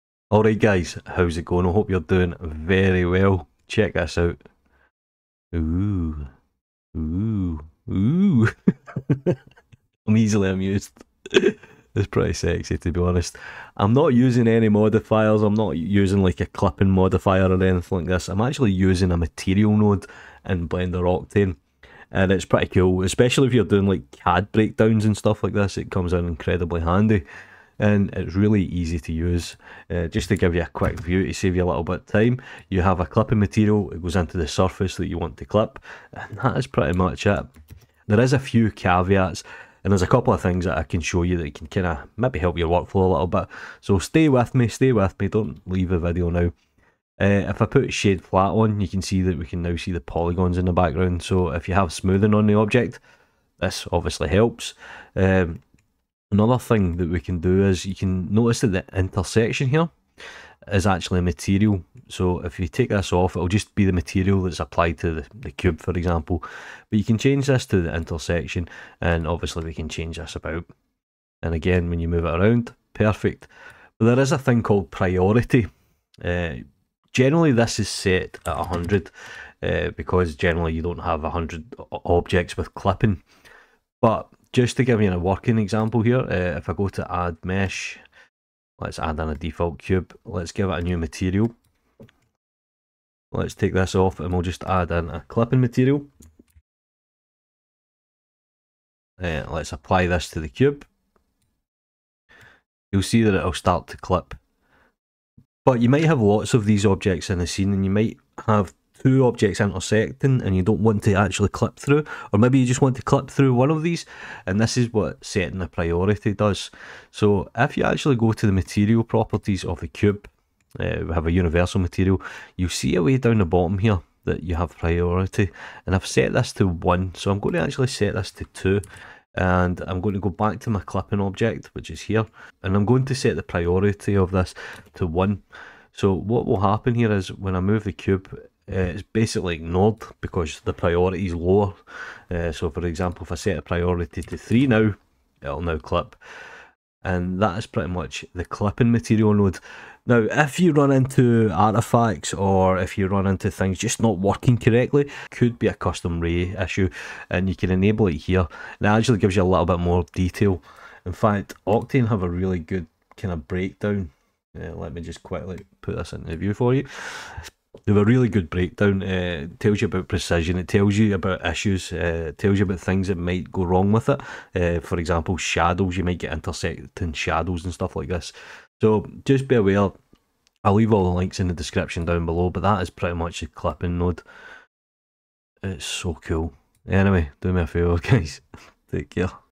Clip Geometry using the Clipping Material Node - Blender Octane
This node is perfect for intersecting geo, perhaps you have a CAD model you want to do breakdowns with, maybe you need a quickway to clip topology, the Clip Material Node for Blender Octane is an ideal use case.

Further Reading